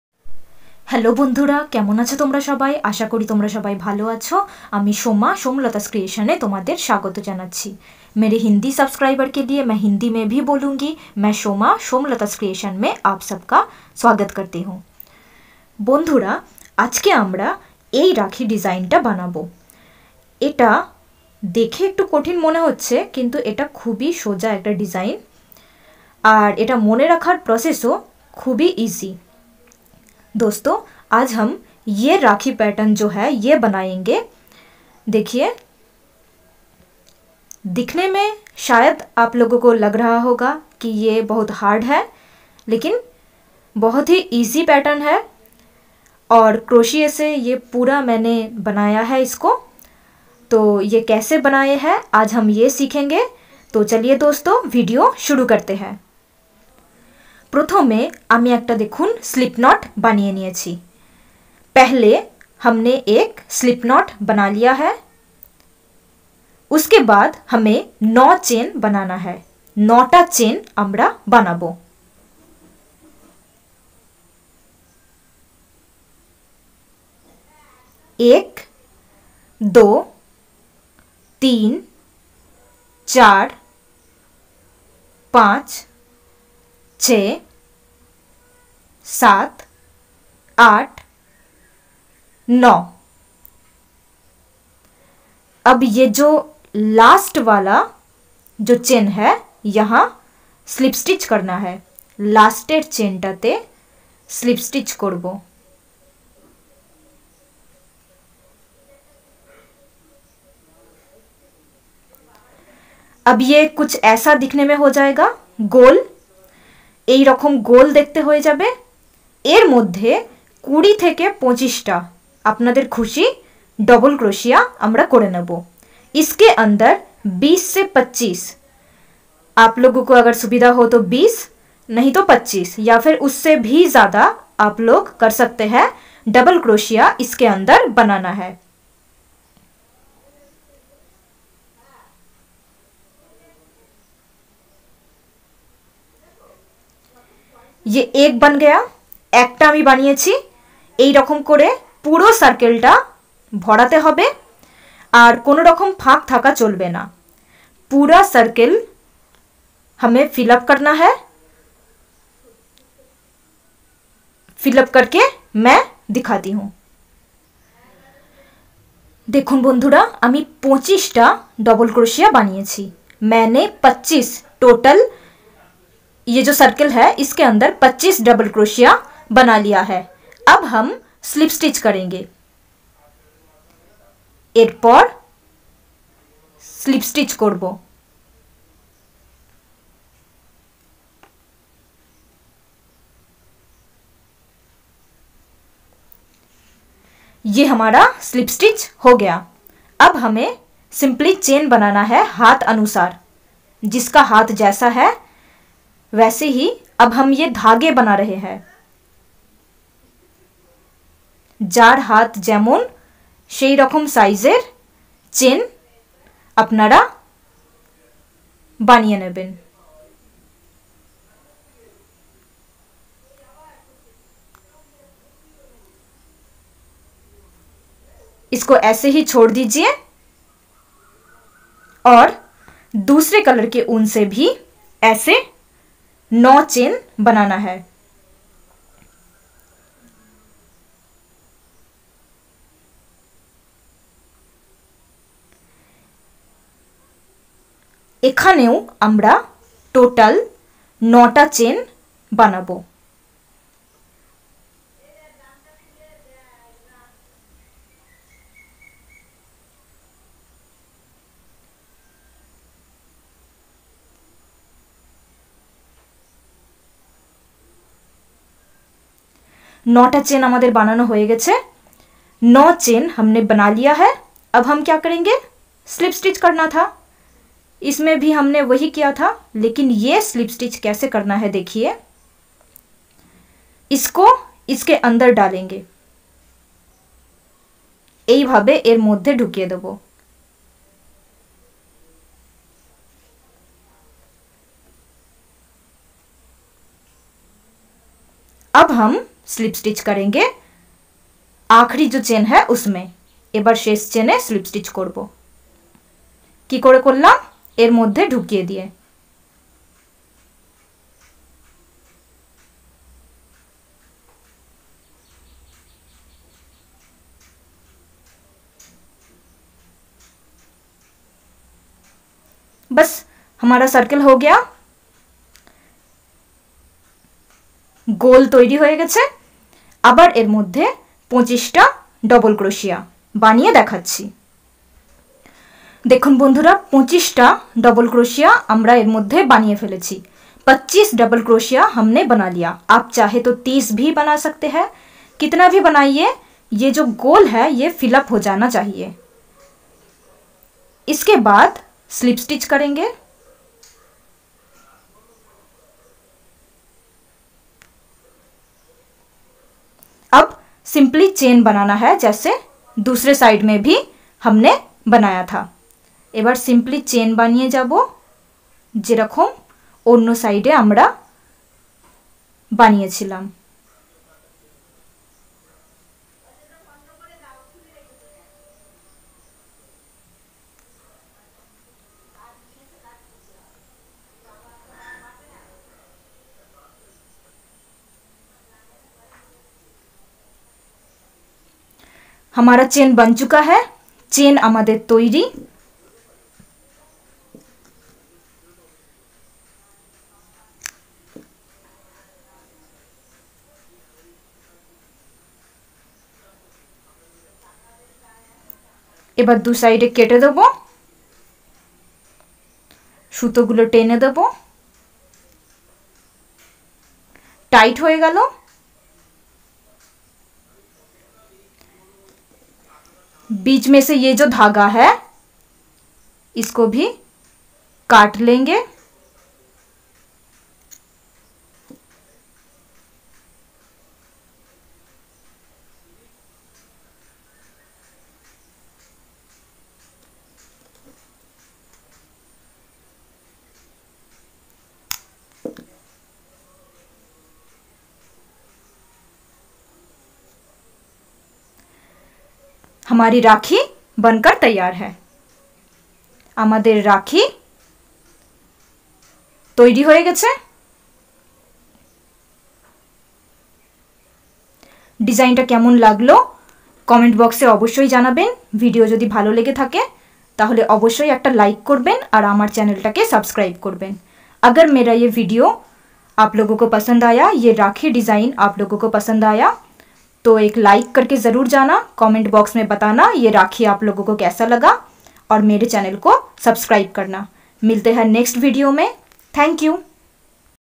Homemade very easy rakhi ( Hindi & Bengali), **रक्षाबंधन स्पेशल**
क्रोशिया की मदद से पहले चैन फिर डबल क्रोशिया बनाकर यह राखी तैयार किया गया है ।

কুরুশের সাহায্যে প্রথমে চেনো তারপর ডবল ক্রোশিয়া বানিয়ে এই রাখি তৈরি করা হয়েছে ।